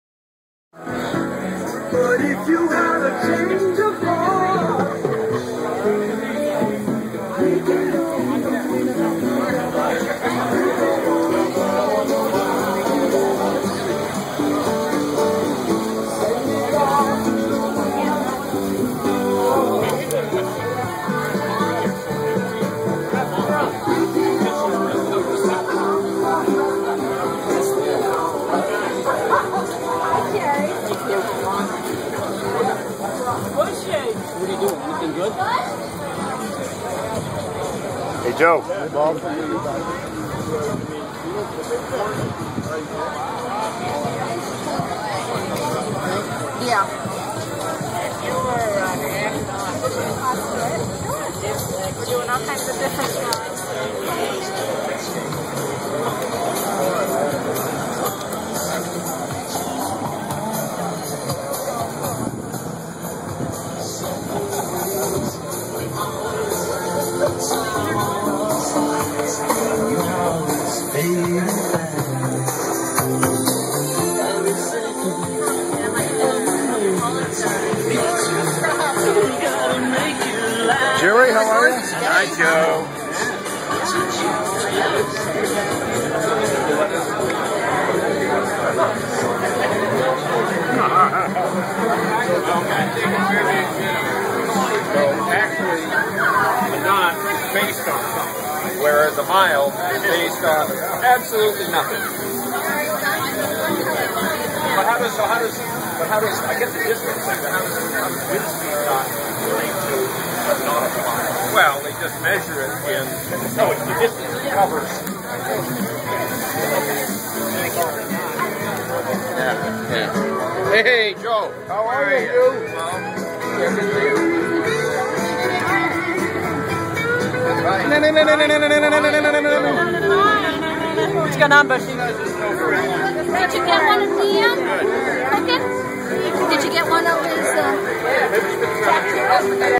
Brattleboro Savings & Loan Community Appreciation Day 2012
A quick walk through Brattleboro Savings & Loan Community Appreciation Day 2012, in the one and only Brattleboro, Vermont!

Under a blue sky and bright sunshine, Brattleboro Savings & Loan held it's annual Community Appreciation Day on Thursday, August 30, 2012 from 11am--1:30pm.

There were hamburgers, hot dogs, veggie burgers, and lots of other goodies served during this time cooked by Dan Yates, members of the Board of Directors, and the Management team. There were also many chances to win some great prizes!

This was a special Community Appreciation Day as Brattleboro Savings & Loan is celebrating their 100th anniversary this year!

*note*
I apologize for the shakiness, it's cell phone-cam footage. I tried the YouTube stabilization fixer, but it made it look a bit distorted, so I undid it.